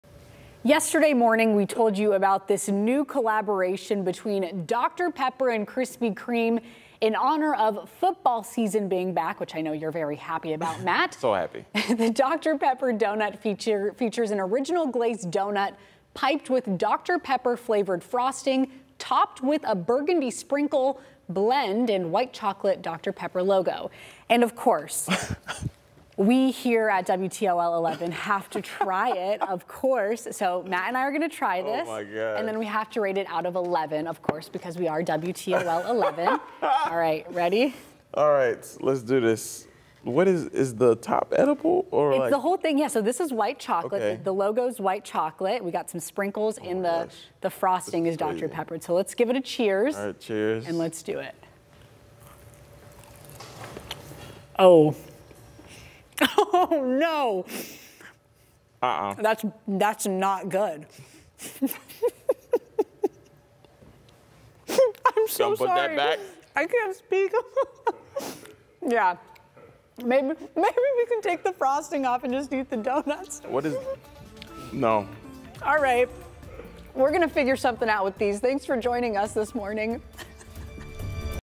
WTOL 11 Weekend Team tries Dr. Pepper Krispy Kreme Donuts
Sophia Perricone and Meteorologist Matt Willoughby get a taste of Krispy Kreme's new Dr. Pepper Donut